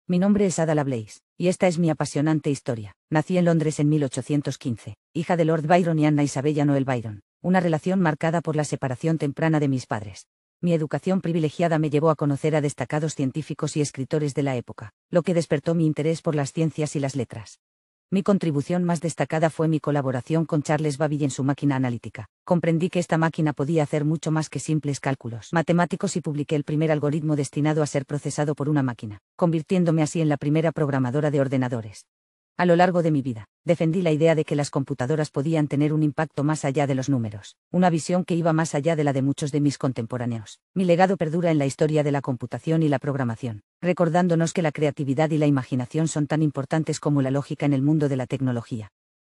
Soy Ada Lovelace y esta es mi historia.
Ada Lovelace (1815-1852) fue una matemática británica y escritora, conocida por sus contribuciones pioneras al campo de la informática. Lovelace es considerada la primera programadora de la historia debido a sus trabajos con la Máquina Analítica de Charles Babbage.

En 1843, Ada Lovelace tradujo un artículo del matemático italiano Luigi Federico Menabrea sobre la Máquina Analítica y agregó sus propias anotaciones, incluyendo el primer algoritmo destinado a ser procesado por una máquina. Este algoritmo se considera el primer programa de computadora, lo que hace de Ada Lovelace una figura fundamental en la historia de la informática.

Su visión de las posibilidades de las máquinas computadoras fue adelantada a su tiempo y su legado perdura como un símbolo de la contribución de las mujeres a la ciencia y la tecnología. El "Día Ada Lovelace" se celebra el 13 de octubre en su honor para promover la participación de las mujeres en la tecnología y la informática.